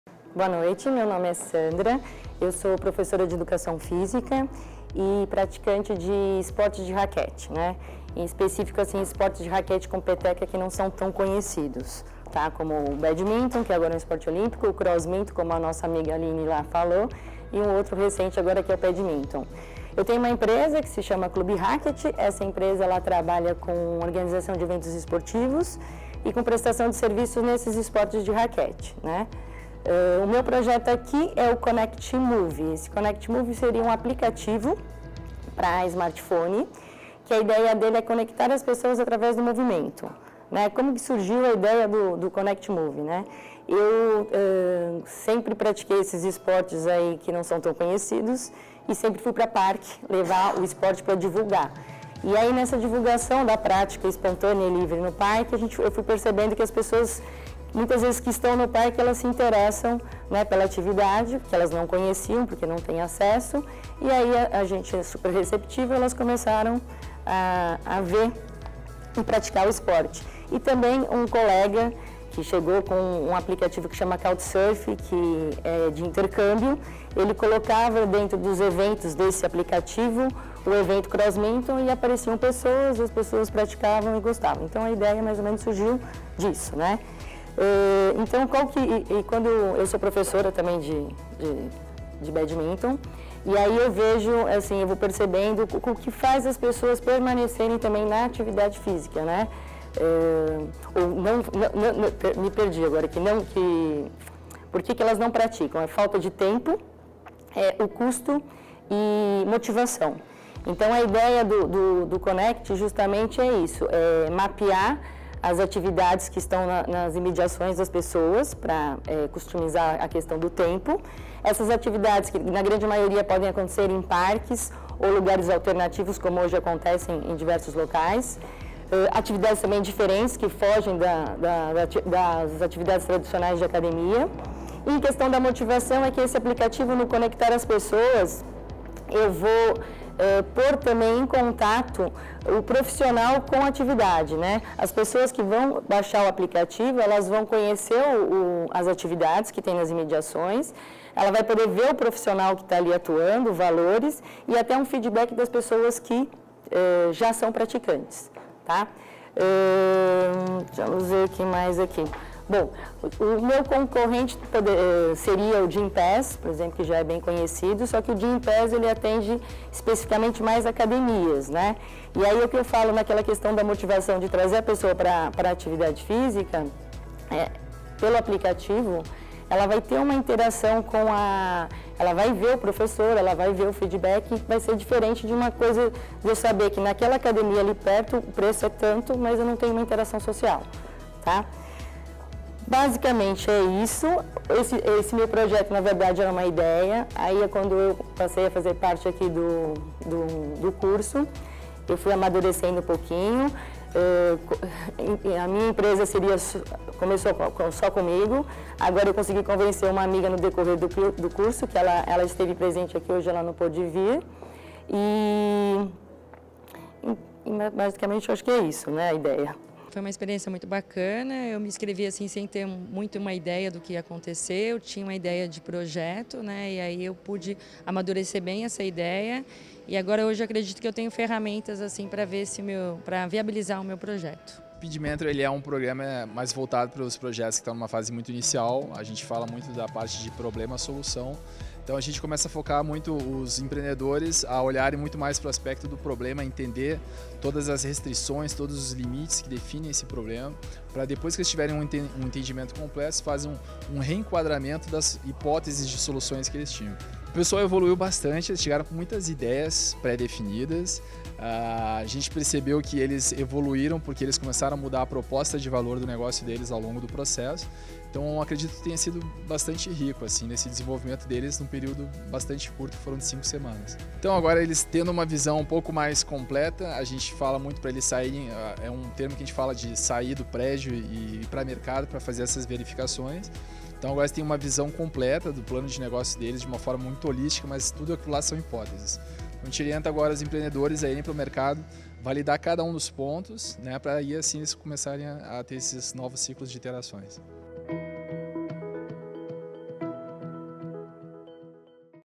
Empreendedora apresenta ideia de aplicativo baseado em conectividade por movimento e esporte
Conheça o Speed Mentoring, da Escola de Negócios deu Sebrae-SP, que é um programa focado em empresas em fase inicial. Com o programa, os empreendedores tem suporte para resolução de diversos problemas enfrentados por empresas iniciantes. Veja no vídeo o depoimento de Dario Henke, consultor do Sebrae-SP, e a apresentação de Sandra Surpreso, falando sobre o ConectMove e como a Escola de Negócios o auxiliou.